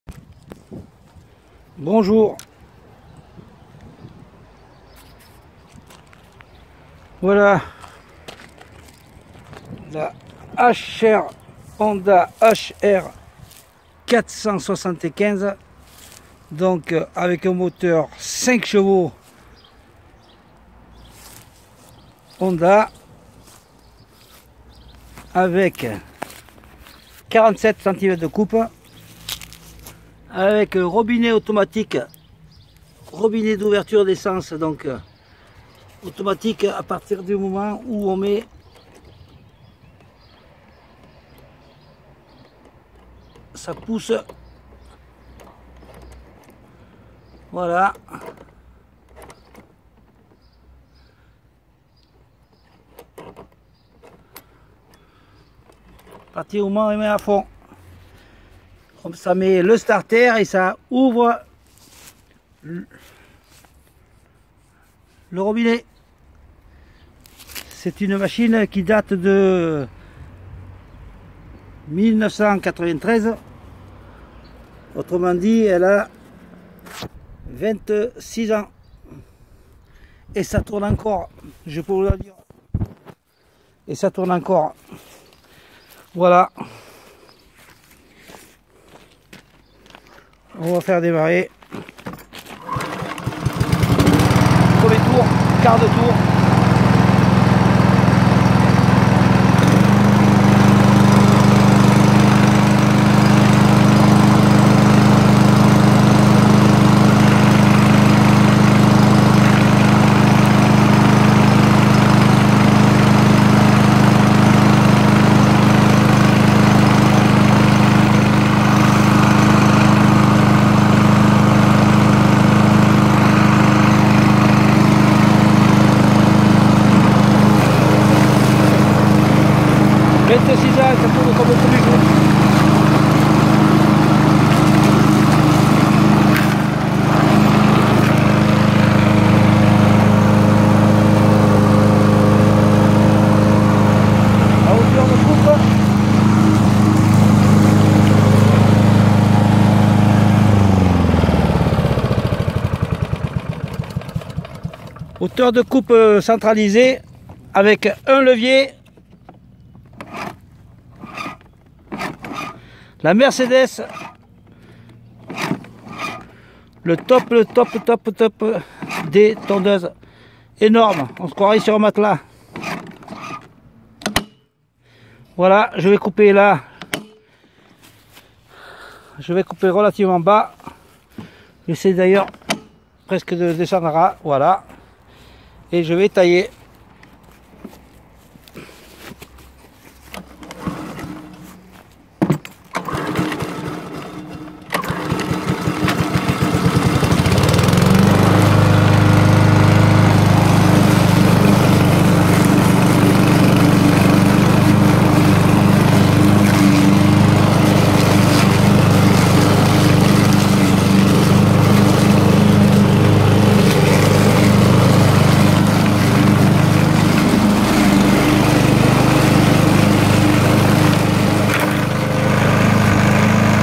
TONDEUSE HONDA HRB 475 . 26 ans déjà!
Bonjour.HONDA HRB 475 veut tout simplement dire  H pour HONDA, R pour tractée et B pour embrayage de lame.Quant à 475,il faut sous entendre 47 cm de carter de coupe et  5 HP de puissance de moteur comme je le précise dans la vidéo.(GXV 140 de 135 cm3) Le soleil est de retour .Profitons en .